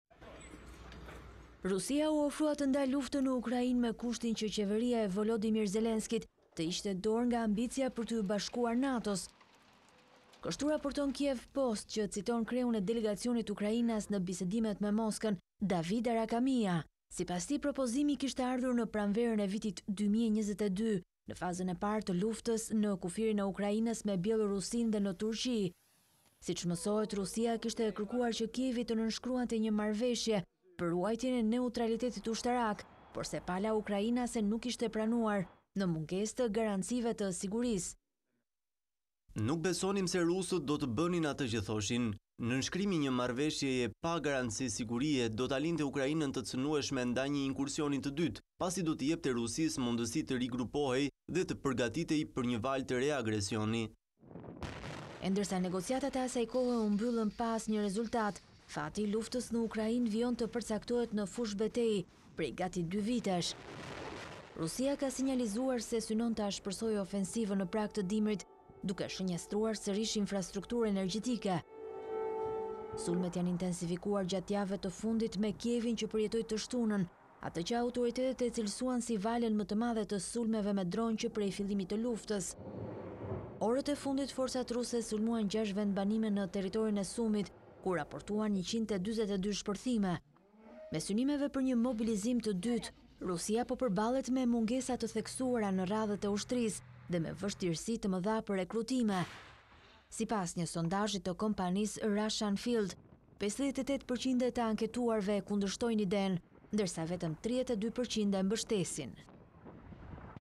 Rusia u ofrua të ndalë luftën, Putin nuk po rekruton dot trupa për ofensivën dimërore
Rusia u ofrua të ndalë luftën në Ukrainë me kushtin që qeveria ukrainase të hiqte dorë nga ambicia për t’iu bashkuar NATO-s. Kështu raporton “Kyiv Post”, që citon kreun e delegacionit Ukrainas në bisedimet me Moskën, David Arakhamia.